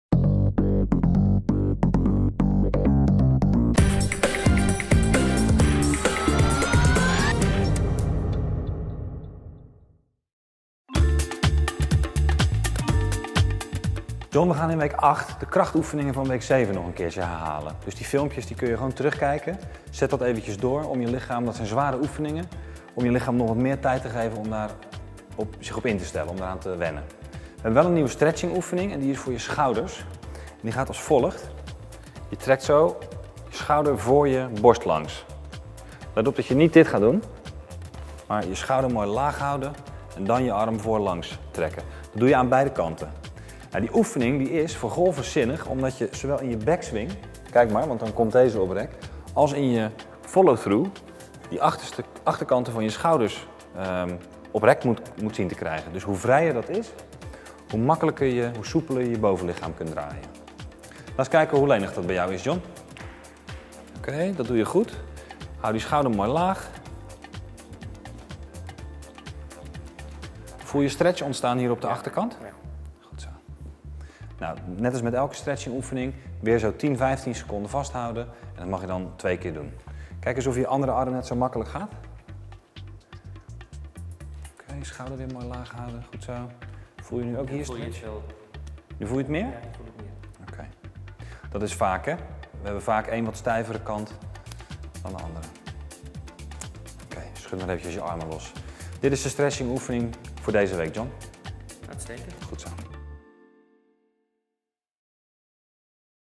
Golf.nl Swingprogramma - Week 8: Fitness oefeningen
Golf.nl presenteert een reeks instructievideo's die je klaarmaken voor het nieuwe golfseizoen. Week 8: Fitness